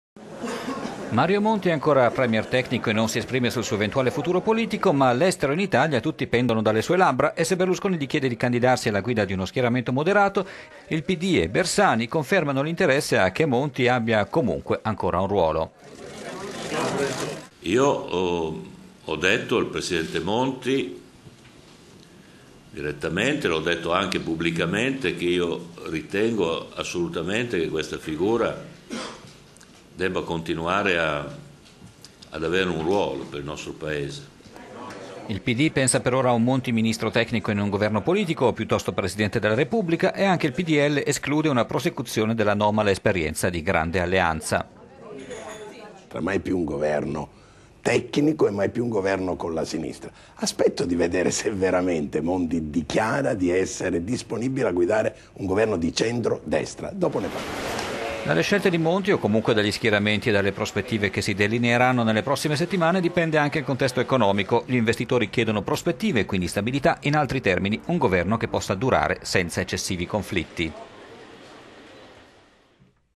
Italia, UE, borse: tutti in attesa delle decisioni di Monti
Mario Monti è ancora premier tecnico e non si esprime sul suo eventuale futuro politico, ma all'estero e in Italia sembra che tutti pendano dalle sue labbra.    E se Berlusconi gli chiede di candidarsi alla guida di uno  schieramento "moderato", il PD e Bersani confermano l'interesse a che Monti abbia comunque un ruolo

Pierluigi Bersani:
"Io ho detto al Presidente Monti direttamente, e l'ho detto anche pubblicamente, che io ritengo assolutamente che questa figura debba continuare ad avere un ruolo, per il nostro Paese"

Il PD pensa per ora a un Monti ministro tecnico in un Governo politico, o piuttosto Presidente della Repubblica, e anche il PdL esclude una prosecuzione dell'anomala esperienza di grande alleanza:

Ignazio La Russa:
"Mai più un governo tecnico e mai più un governo con la sinistra.  Aspetto di vedere se veramente Monti dichiara di essere disponibile a guidare un governo di centro-destra.  E dopo ne parliamo".

Dalle scelte di Monti, o comunque dagli schieramenti e dalle prospettive che si delineeranno nelle prossime settimane, dipende anche il contesto economico: gli investitori chiedono prospettive, e quindi stabilità.  In altri termini, un Governo che possa durare senza eccessivi conflitti.